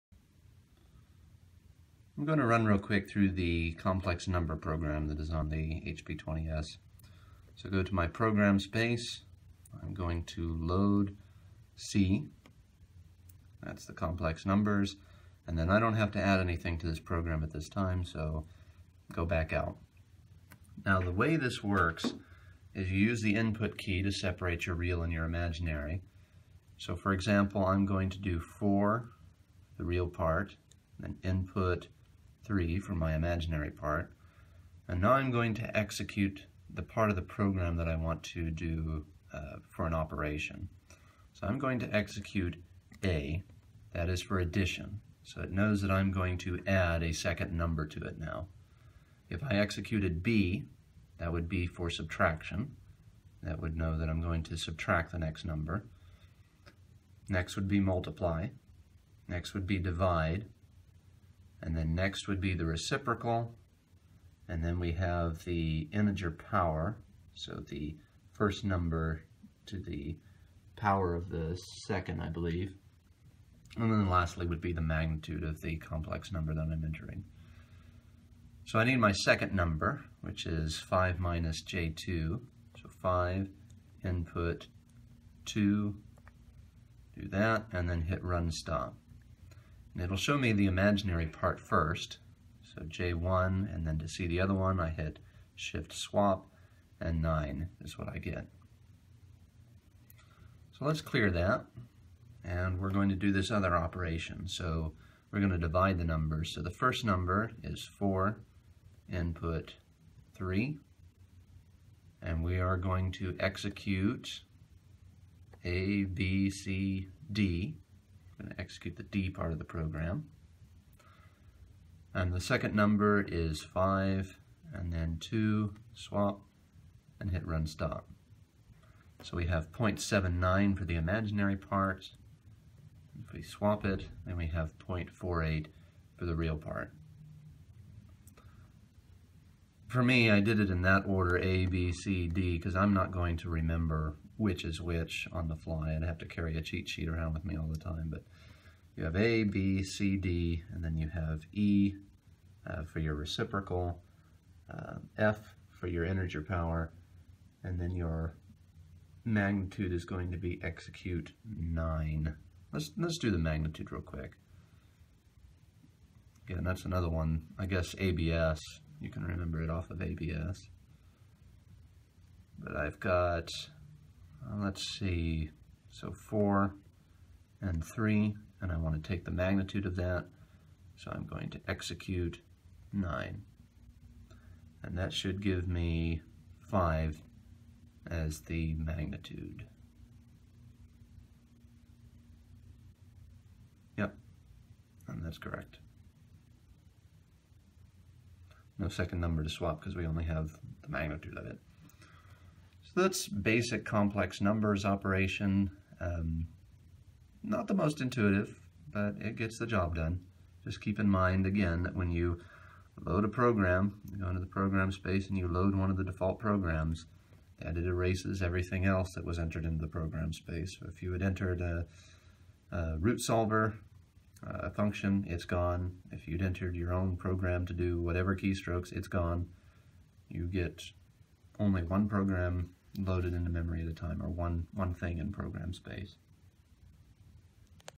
HP 20S Built In Program 3 - Complex Numbers
Basic introduction to the built-in program for handling complex numbers.